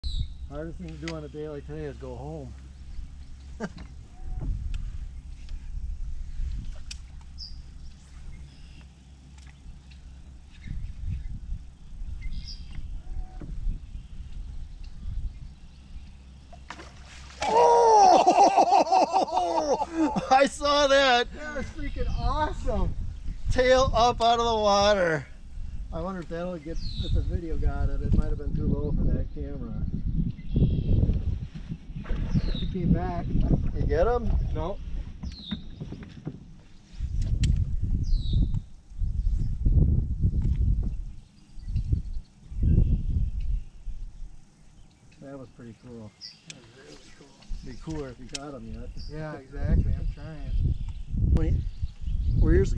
Pike chases lure out of the water
Pike comes out of the water after the lure